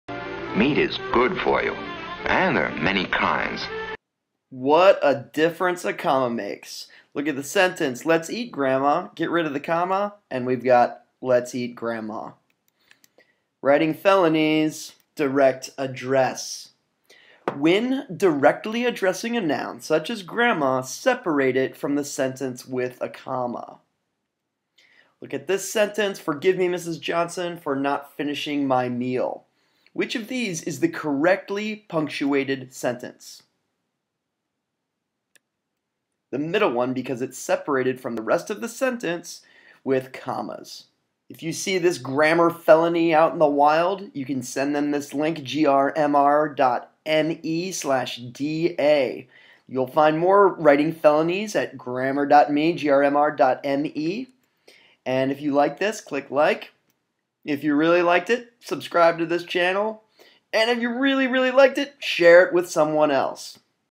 Writing Felonies: Direct Address
How to properly punctuate a direct address.